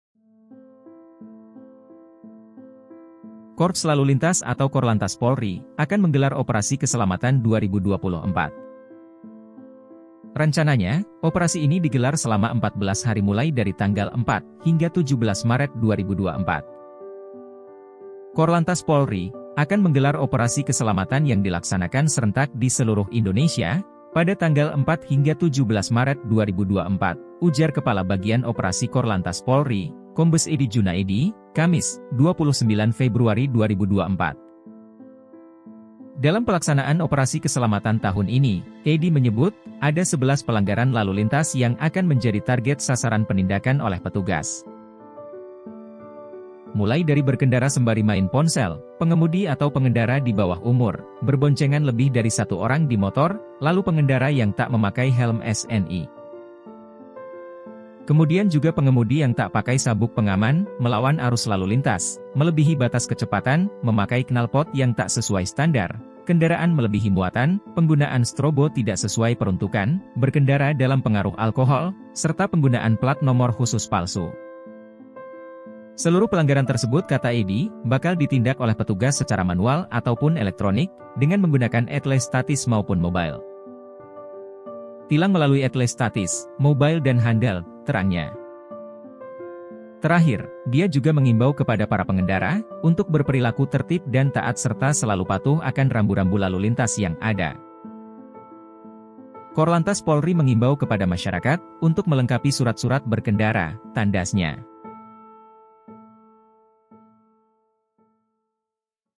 Polri Bakal Gelar Operasi Keselamatan 2024, Ini Jenis Pelanggaran yang Bakal Ditindak
Jubir TV - Korps Lalu Lintas (Korlantas) Polri akan menggelar Operasi Keselamatan 2024.

Rencananya operasi ini digelar selama 14 hari mulai dari tanggal 4 hingga 17 Maret 2024.

"Korlantas Polri akan menggelar Operasi Keselamatan yang dilaksanakan serentak di seluruh Indonesia pada tanggal 4-17 Maret 2024," ujar Kepala Bagian Operasi (Kabag Ops) Korlantas Polri Kombes Eddy Djunaedi, Kamis (29/2/2024).

Dalam pelaksanaan Operasi Keselamatan tahun ini, Eddy menyebut ada sebelas pelanggaran lalu lintas yang akan menjadi target sasaran penindakan oleh petugas.

Mulai dari berkendara sembari main ponsel, pengemudi atau pengendara di bawah umur, berboncengan lebih dari satu orang di motor, lalu pengendara yang tak memakai helm SNI.

Kemudian juga pengemudi yang tak pakai sabuk pengaman, melawan arus lalu lintas, melebihi batas kecepatan, memakai knalpot yang tak sesuai standar, kendaraan melebihi muatan, penggunaan strobo tidak sesuai peruntukan, berkendara dalam pengaruh alkohol, serta penggunaan plat nomor khusus palsu.

Seluruh pelanggaran tersebut kata Eddy, bakal ditindak oleh petugas secara manual ataupun elektronik dengan menggunakan ETLE statis maupun mobile.

"Tilang melalui ETLE statis, mobile dan handheld," terangnya.

Terakhir, dia juga mengimbau kepada para pengendara untuk berperilaku tertib dan taat serta selalu patuh akan rambu-rambu lalu lintas yang ada.

"Korlantas Polri mengimbau kepada masyarakat untuk melengkapi surat-surat berkendara," tandasnya.